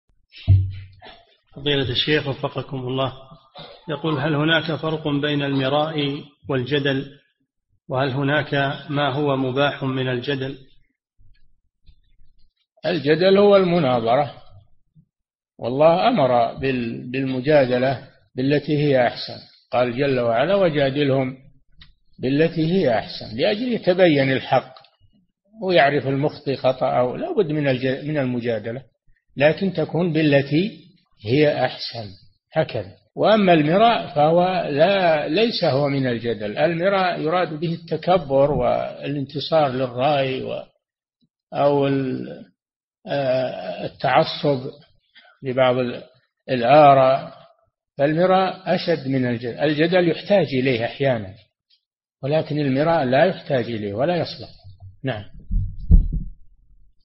[281 -308] هل هناك فرقٌ بين المراء والجدل، وهل هناك ما هو مباحٌ من الجدل؟ - الشيخ صالح الفوزان
-[281 -308] السؤال: هل هناك فرقٌ بين المراء والجدل، وهل هناك ما هو مباحٌ من الجدل؟
الجواب: الجدل هو المناظرة، والله أمر بالمجادلة بالتي هي أحسن، قال جل وعلا: ﴿ وَجَادِلْهُمْ بِالَّتِي هِيَ أَحْسَنُ﴾ [النحل:125]، لأجل يتبين الحق، ويعرف المُخطئ خطأه لابد من المُجادلة، لكن تكون بالتي هي أحسن.. هكذا، وأما المراء فهو ليس هو من الجدل المِراء يُراد به التكبر والانتصار للرأي، أو التعصب لبعض الآراء، فالمِراء أشد من الجدل، الجدل يُحتاج إليه أحيانًا، ولكن المِراء لا يحتاج إليه ولا يصلح.
للاشتراك في قناة الشيخ صالح الفوزان

للاستفسار والإقتراحات والدعم
رئيس الفريق
لمشروع كبار العلماء